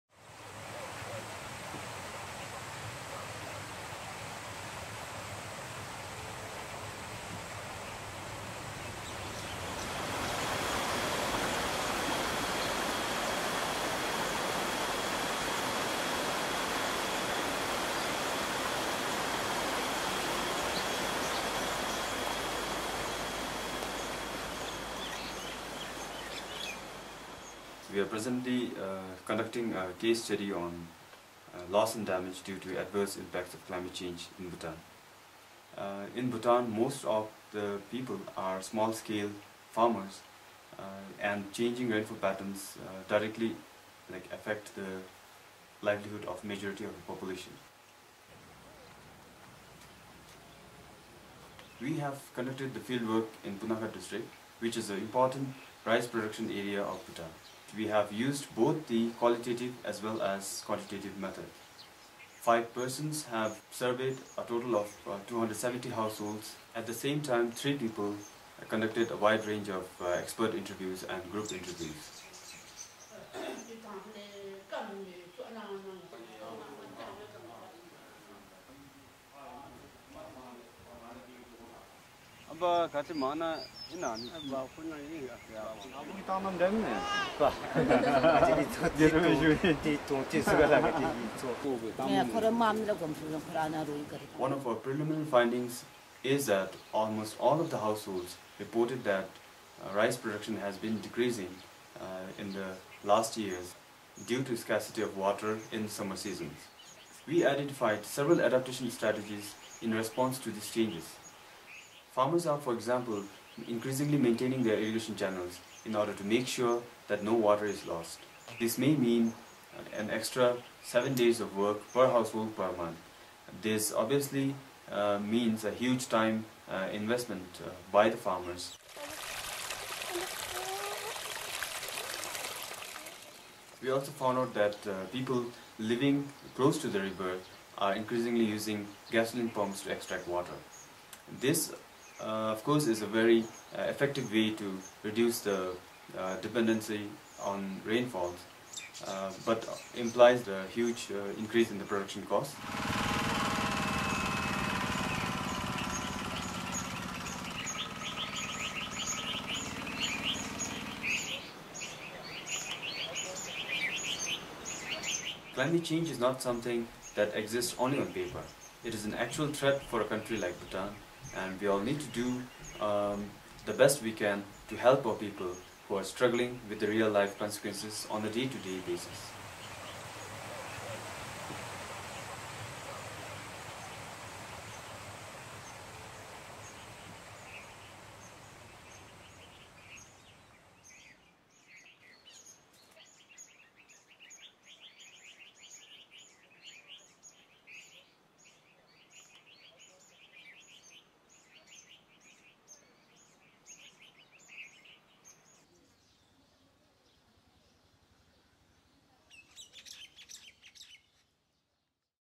Loss and Damage case study fieldwork, Bhutan
Video with researcher Norbu Wangdi sharing early findings of the Bhutan case study of CDKN's Loss and Damage in Vulnerable Countries Initiative. The video was recorded during the fieldwork in June 2012

Focus: The impact of changing monsoon patterns on rice production
Research area: Punakha District

Findings: In Punakha District, late onset of monsoon rains and lower total amounts of monsoon rain decrease the availability of water for paddy rice irrigation. Farmers try to adapt by changing water-sharing arrangements, intensifying irrigation channel maintenance, and by shifting from rice to lower-yielding, rain-fed crops. A small minority can afford to buy gasoline pumps. For 87% of the survey respondents (N=273), the measures are not enough and/or entail extra costs. In addition, some adaptation measures are reported to cause conflicts between households and villages.

Camera, interview and editing: Koen Kusters